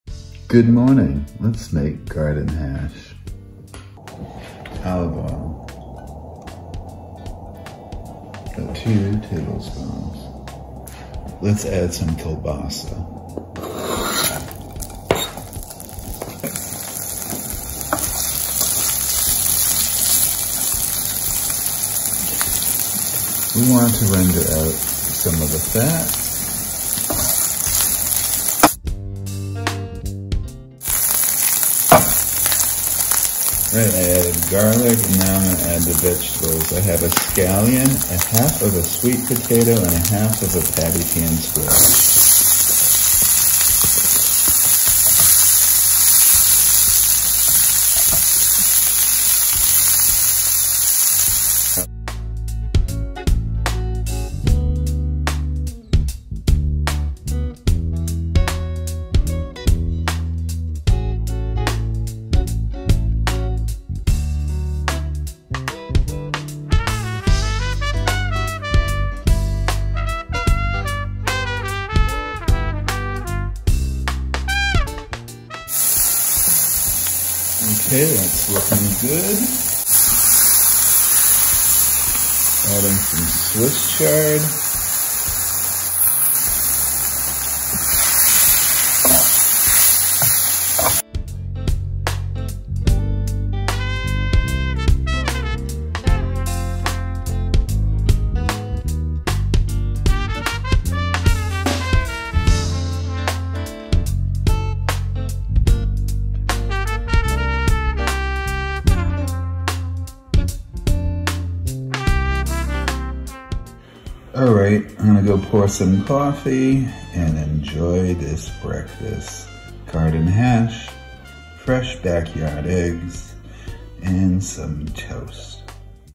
Let’s Make Garden Hash!
This video is meant to show the method, rather than a recipe to follow.  Make it your own . You can use whatever breakfast protein you have on hand, and switch out whatever veggies you have that make sense. Are you vegan, switch out to mushrooms and/or beans. I hope you’ll try it.

I used a half of a baked sweet potato, I didn’t mention it was cooked in the video.